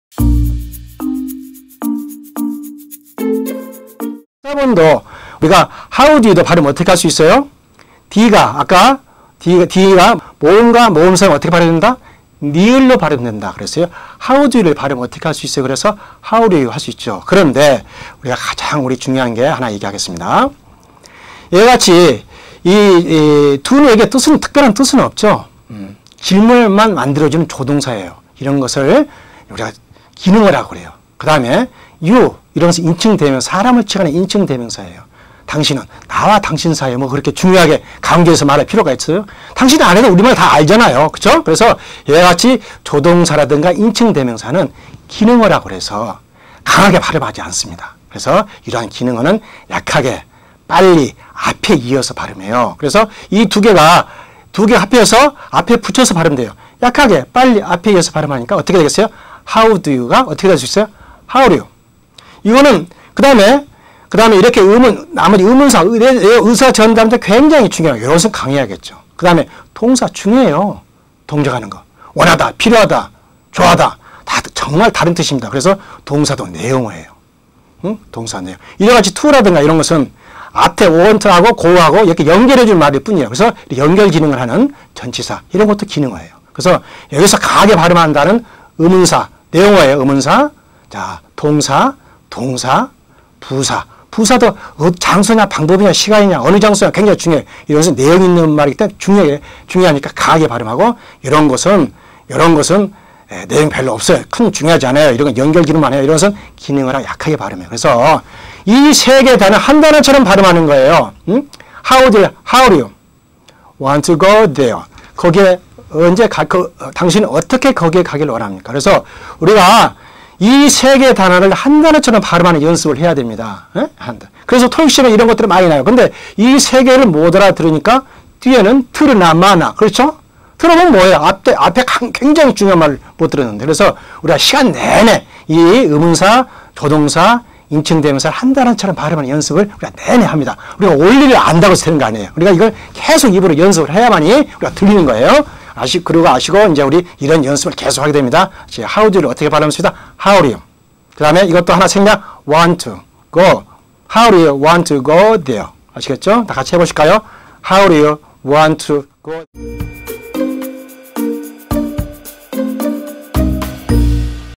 [영어벙어리속성탈출] 영어문법에 맞는 영문장 만들기!
영어문법에 맞는 영문장을 만들어보자.

지금 바로 PLAY

영어공부,영어문제,영어회화,영어,영어인강,영어강의,영문법,영어벙어리속성탈출,영어배우기,단골tv